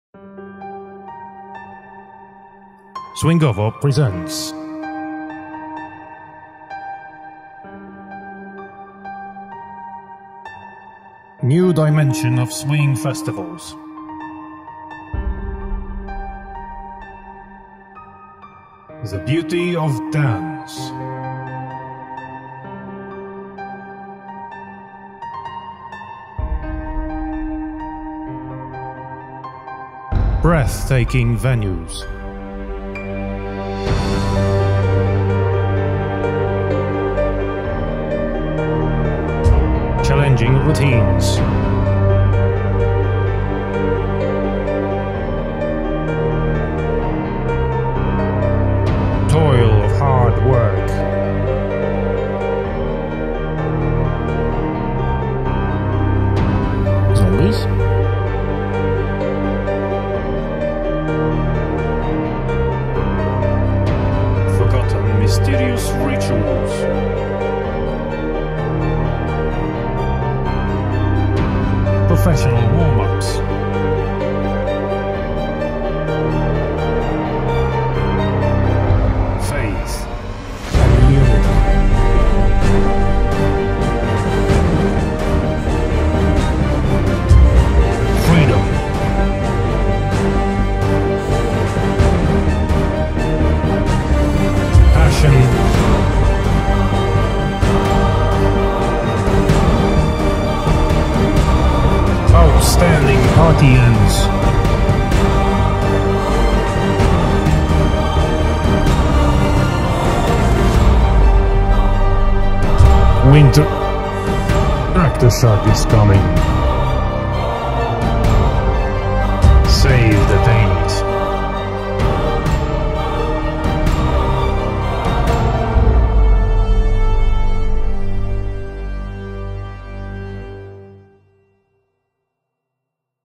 KRAK the SHAG - swing festival trailer made by Swingowo
During Barcelona Shag Festival 2015 we thought that shag festival in Krakow would be a great idea. At that point we were just a group of friends, spending way too much time for swing.

The idea for name - Krak the Shag - hit us during long walks in Barcelona over a cup of delicious wine. The first edition took place last winter. We invited incredible Michał Oblaciński and Katarzyna Dybowska - they did a great work igniting Shag fever in our community.

Shag hit the Kraków dancefloors and the impact was hard! To keep the momentum we decided to make something bigger. Something worth trailer.

Our first international shag&balboa festival "Krak the Shag 2017" is placed in Cracow, Poland. Made by Swingowo, and we are proud of it:)